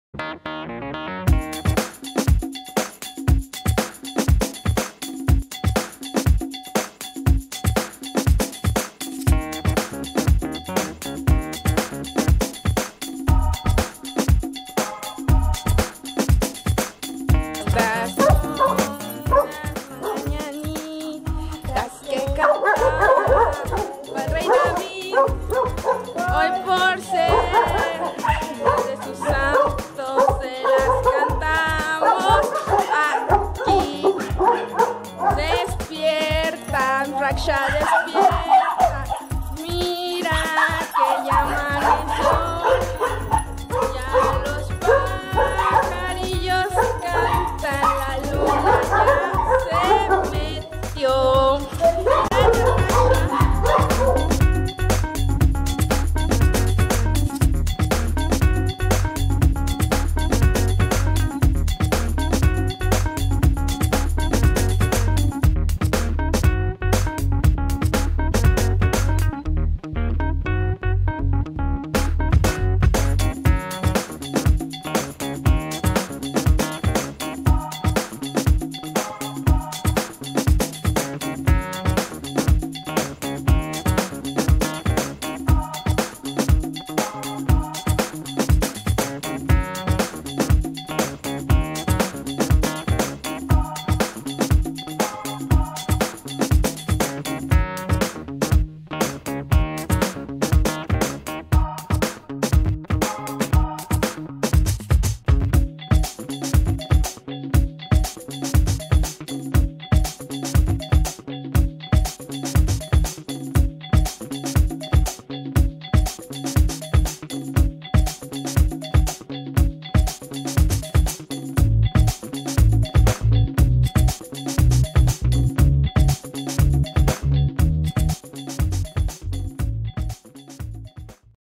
Kinder canino: Pastel de cumpleaños de RAKSHA
Fué el cumpleaños de Raksha y se la pasaron super bien